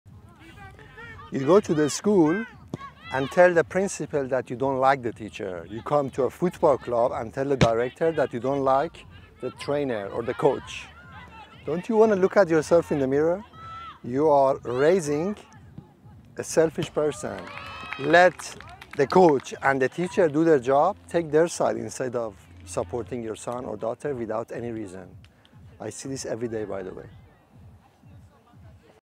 Parents can be dinosaurs too! 🦕
Raising selfish individuals instead of helping the child develop.